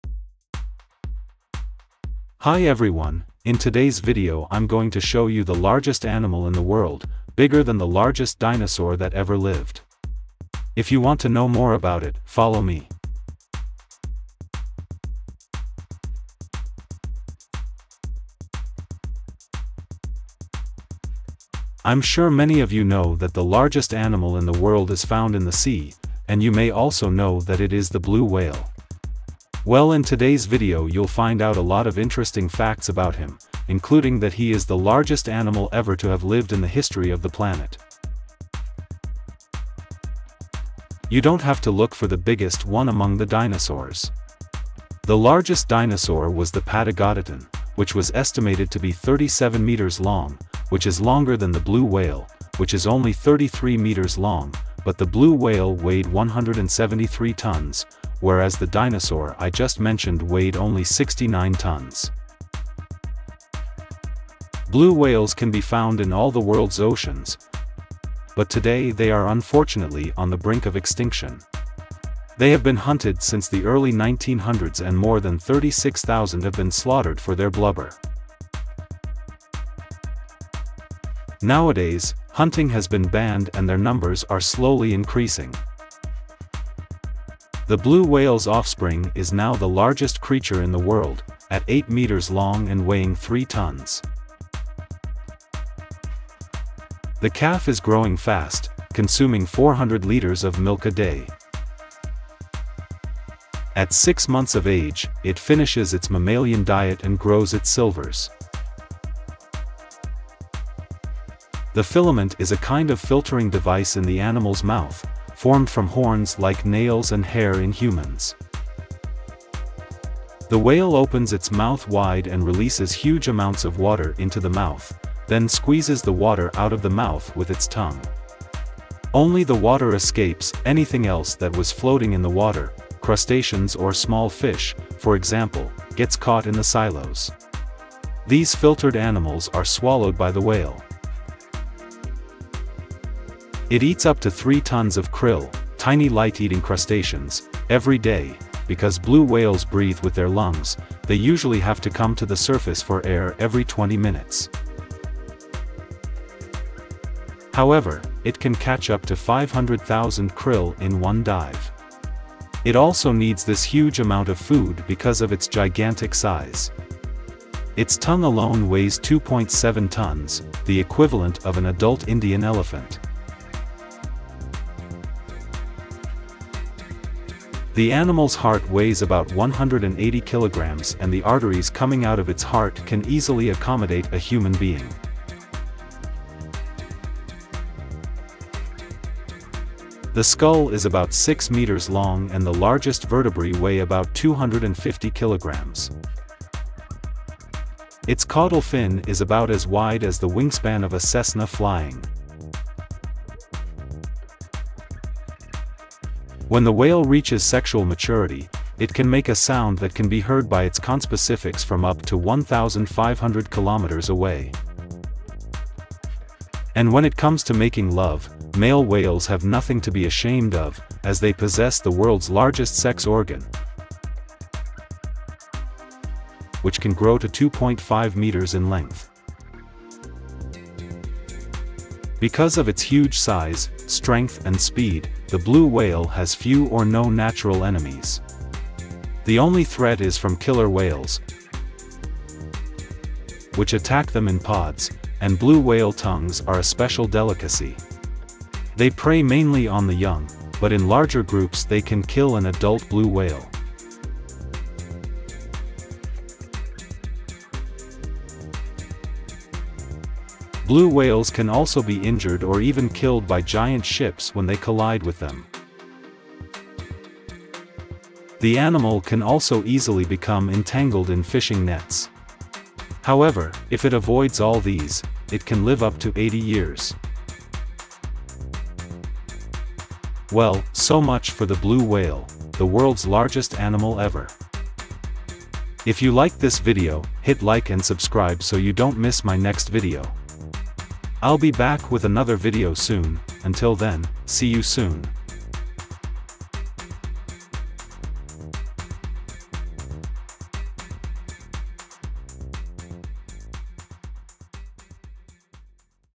The largest animal in the world
In today's video, I'm going to show you the world's largest animal, the blue whale, which is bigger than the largest dinosaur that ever lived.